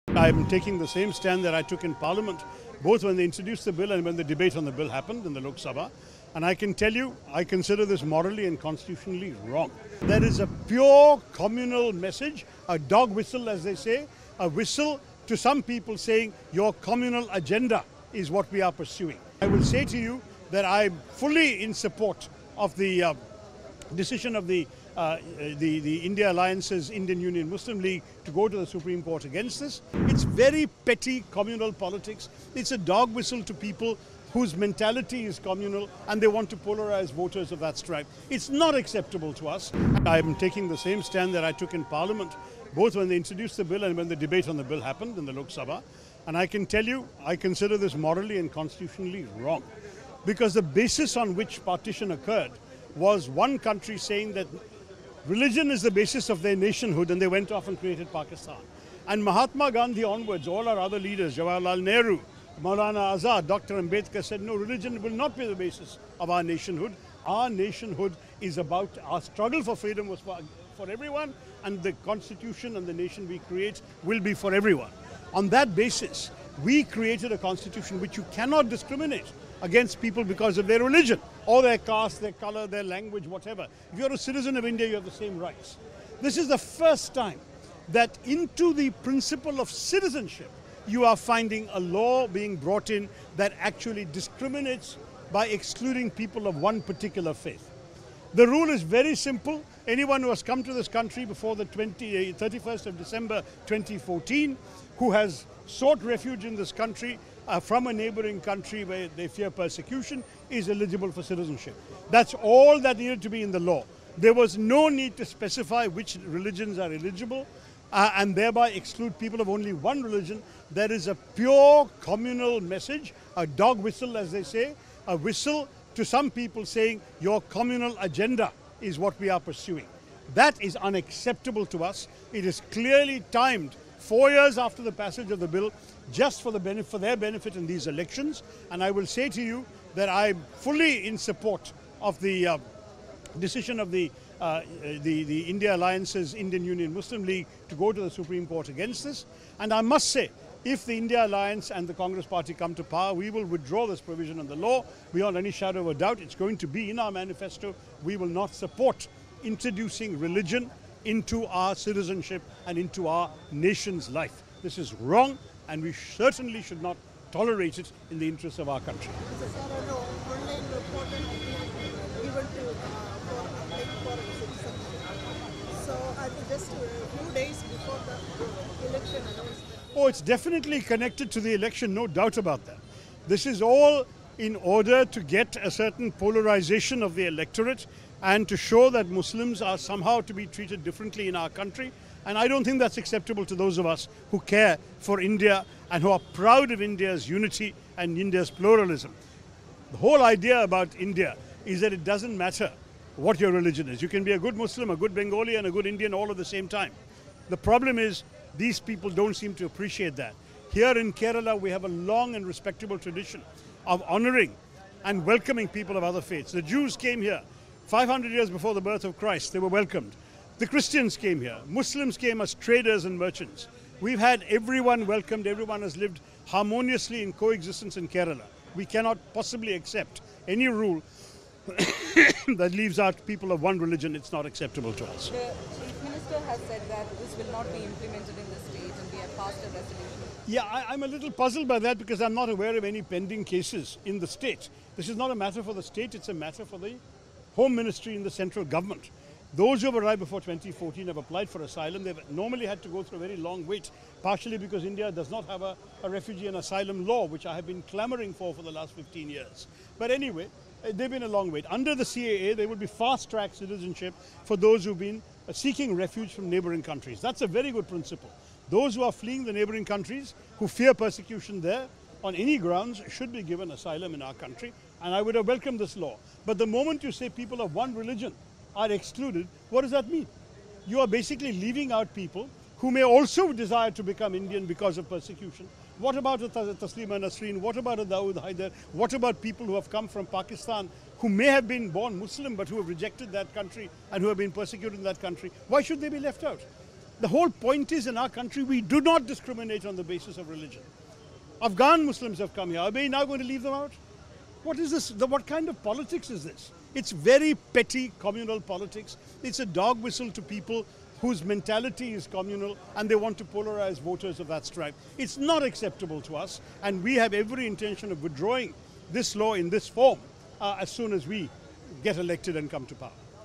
CAA: “Morally, Constitutionally Wrong, Exposes BJP's Communal Agenda..” Shashi Tharoor | Top News
Congress leader Shashi Tharoor opposed the implementation of the Citizenship Amendment Act (CAA), calling it morally and constitutionally wrong. Tharoor supported challenging the CAA in the Supreme Court, expressed backing for the INDIA Alliance's efforts, and pledged to withdraw the provision if Congress comes to power. He criticized the exclusion based on religion and stressed the importance of providing asylum to those fleeing persecution in neighboring countries, regardless of their religion.